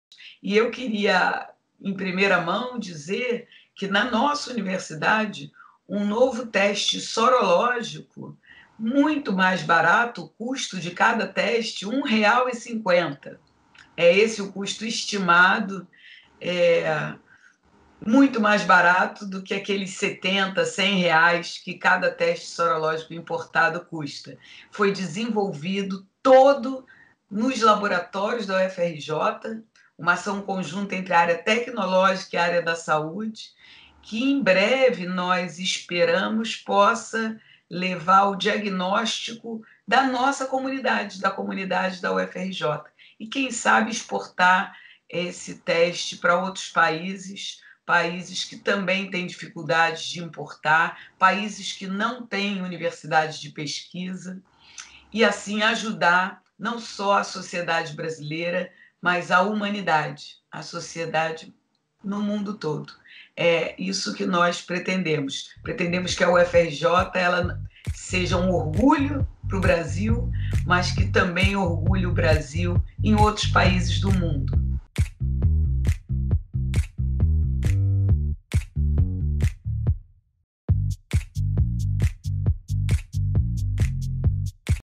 Teste para Covid-19 produzido pela UFRJ
A Reitora Denise Carvalho, em entrevista ao TJ UFRJ, revelou a produção de um novo teste sorológico para detecção de anticorpos para o coronavírus, desenvolvido nos laboratórios da UFRJ. 🦠🧪⁣
Esse teste custará apenas R$1,50 por unidade, valor 60 vezes menor do que o dos testes importados atualmente. De acordo com a Reitora, há perspectiva para exportação do novo teste produzido na Universidade, além do uso em território nacional.⁣
UFRJ no front de combate à Covid-19! Isso é universidade pública! 💪⁣